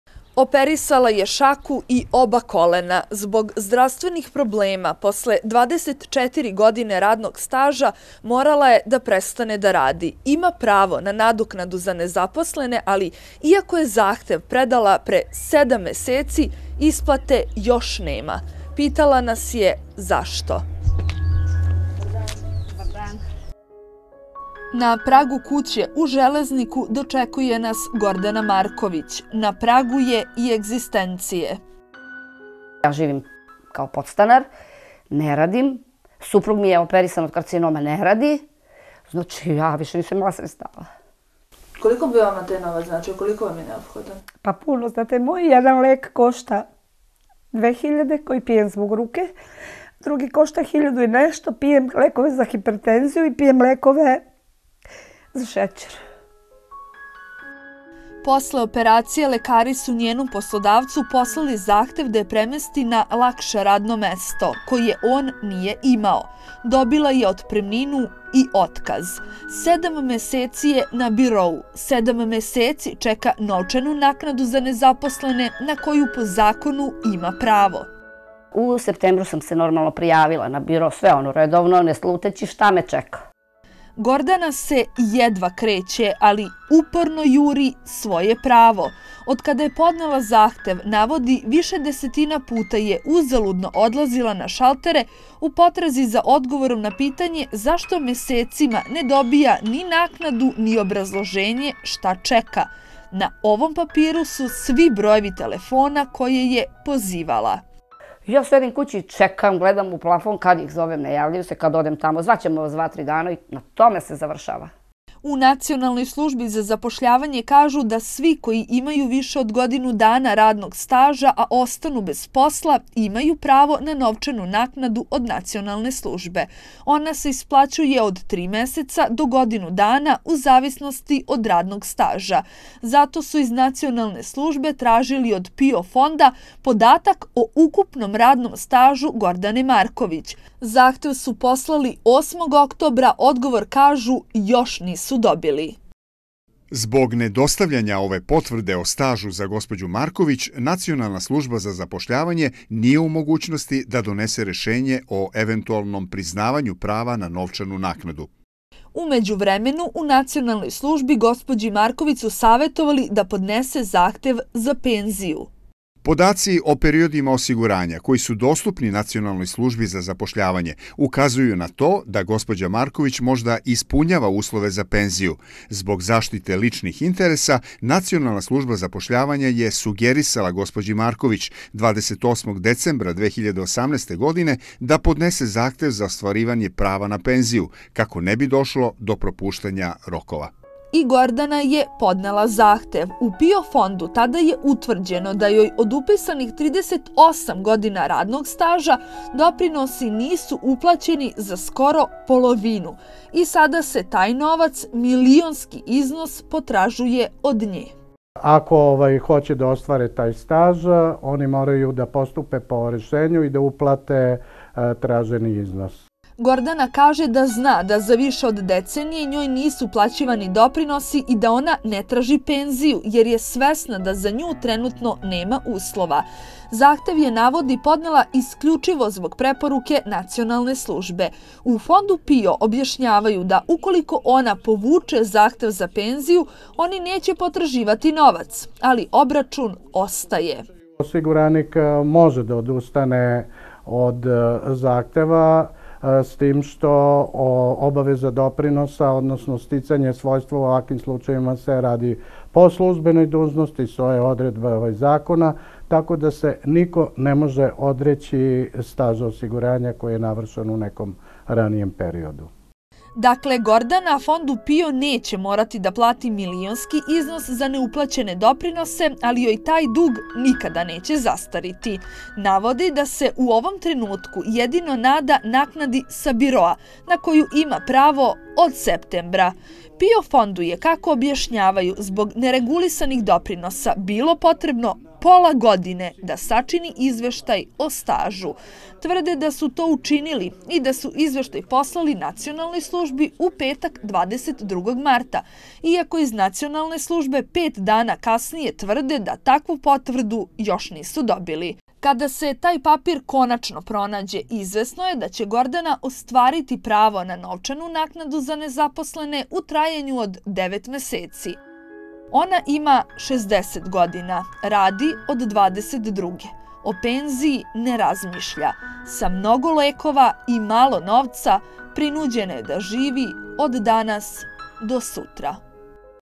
Ostala bez posla, dobila dug umesto nadoknade
Gordana Marković je ostala bez posla i podnela zahtev za novačnu naknadu, a završila je sa zahtevom za penziju i milionskim dugom. U njenom slučaju ništa nije jednostavno a problem već sedam meseci ne može sama da reši.